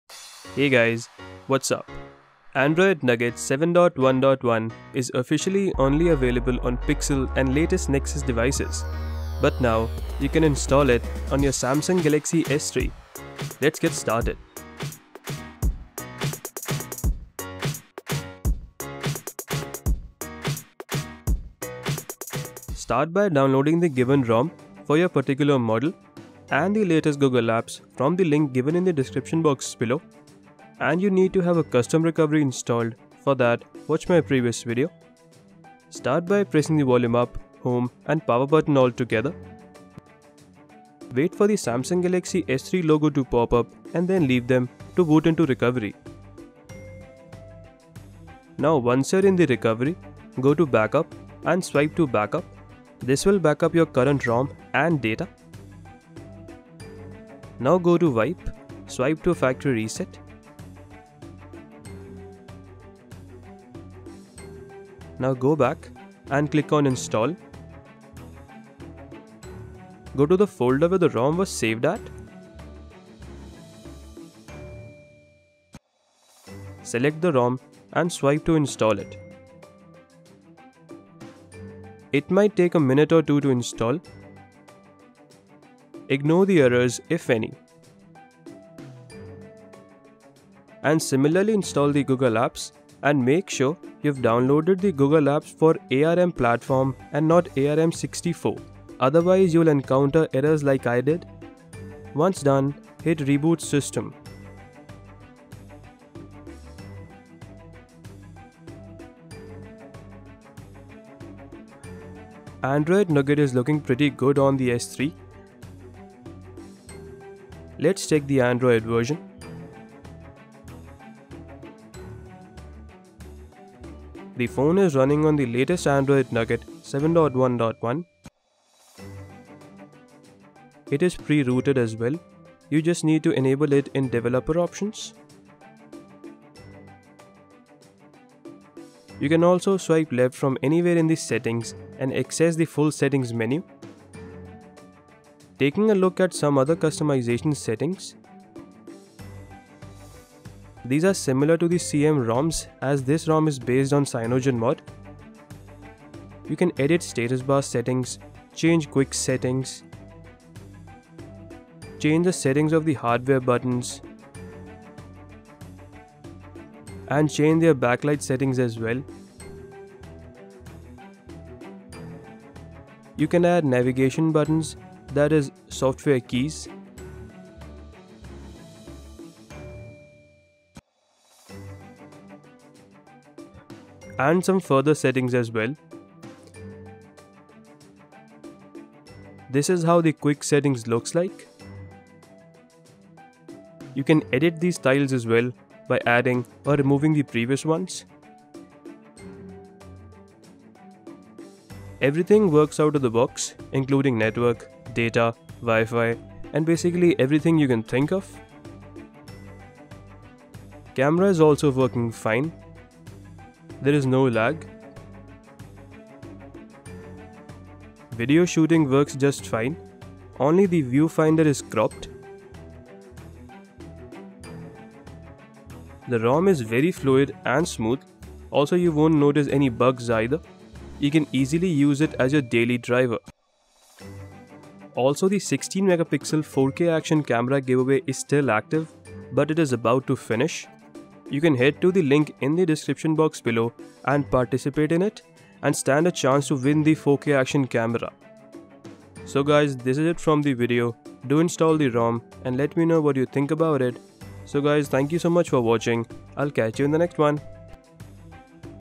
Install Android Nougat(7.1.1) On Samsung Galaxy S3!
Install Android Nougat(7.1.1) On Samsung Galaxy S3 by Tech Tube!.

Save these to SD card & carefully follow instructions to install Galaxy S7 Rom on your Samsung Galaxy S3!
For exciting Deals visit banggood